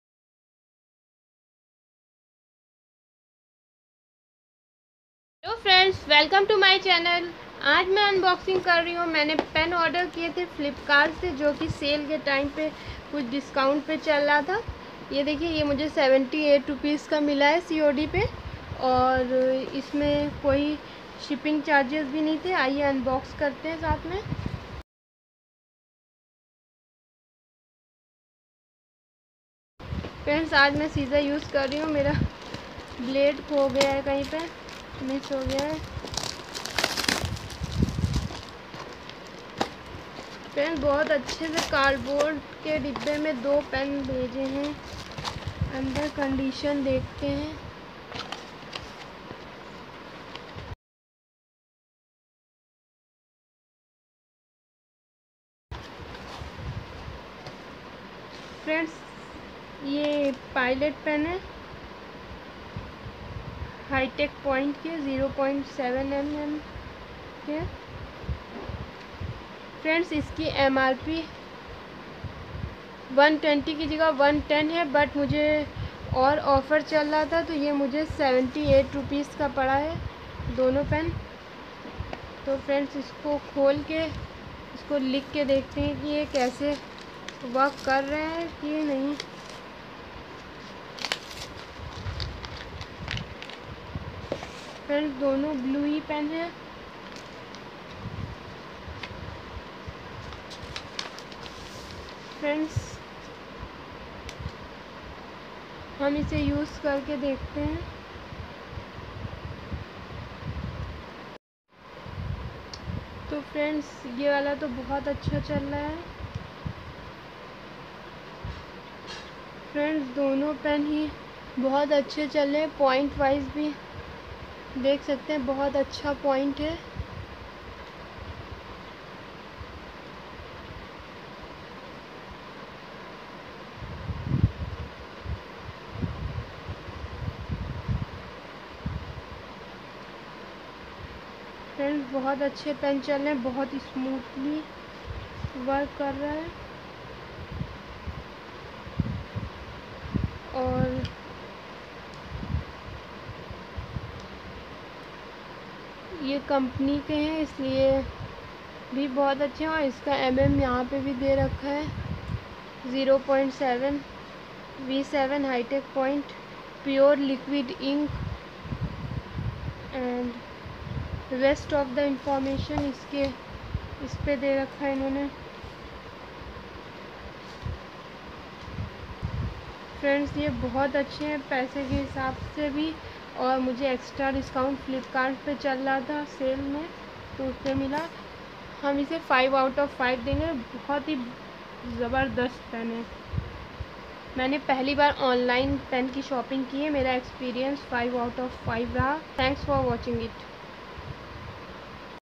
Pen unboxing / review / pilot pen / from flipkart / in Hindi
Pilot pen unboxing and review .
From filpkart .
MRP = 110
After discount = 78 😀😀
Super pens .
Rating from my side will be 5/5

Any queries regardingthis video pleasecomment below.